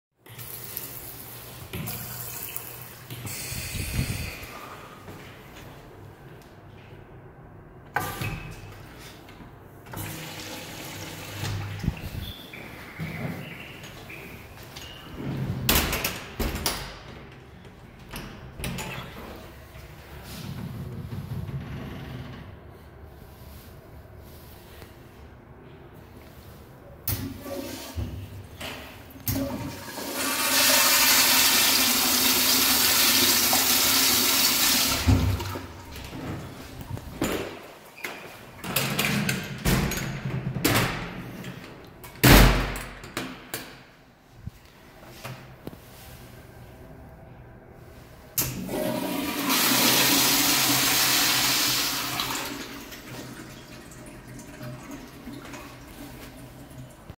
645. Burlington's restroom
Film at wonderland of Americas mall
In San Antonio TX
This restroom is ok
6/10